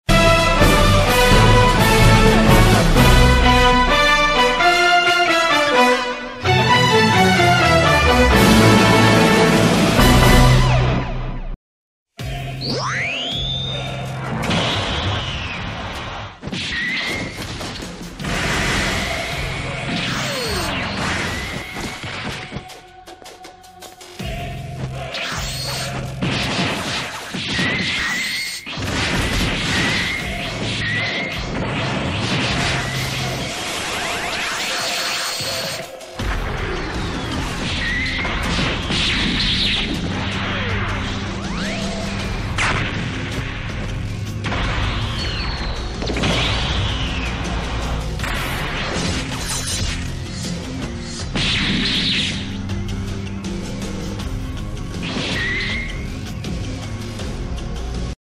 Goku black vs Vegeta(part1)
Pivotsticknodes back again with another epic video!!this time Goku black fights the Saiyan prince Vegeta 1 on 1.
This is the beginning of something extraordinary.